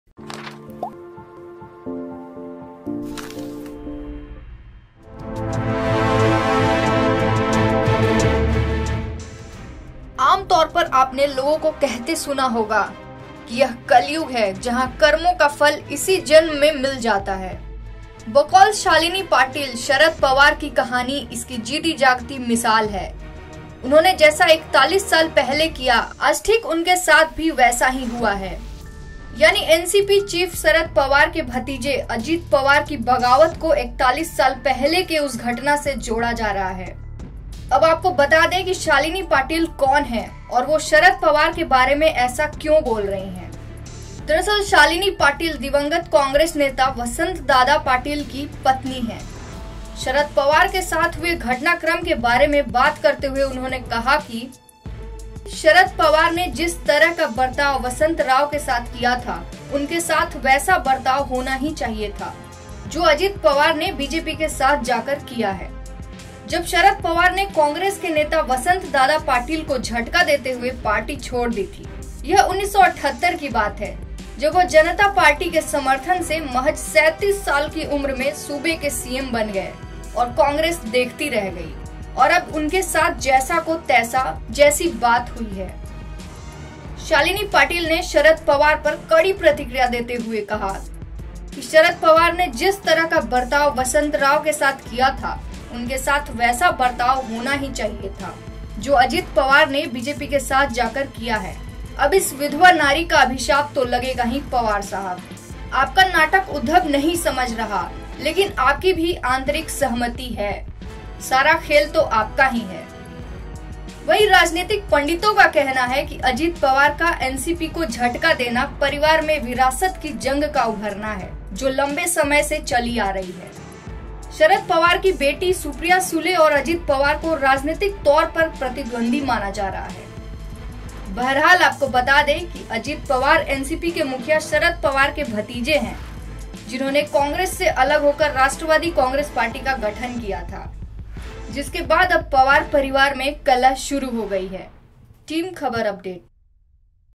Sharad Pawar के साथ जो हुआ, 41 साल पहले उन्होंने Vasant Dada के साथ किया था | Khabar Update
कांग्रेस नेता वसंत दादा पाटील की पत्नी शालिनी पाटील ने शरद पवार पर कड़ी प्रतिक्रिया देते हुए कहा कि "शरद पवार ने जिस तरह का बर्ताव वसंत राव के साथ किया था, उनके साथ वैसा बर्ताव होना ही चाहिए था, जो अजित पवार ने बीजेपी के साथ जाकर किया है. अब इस बिधवा नारी का...आगे जानने के लिए देखिए ये रिपोर्ट-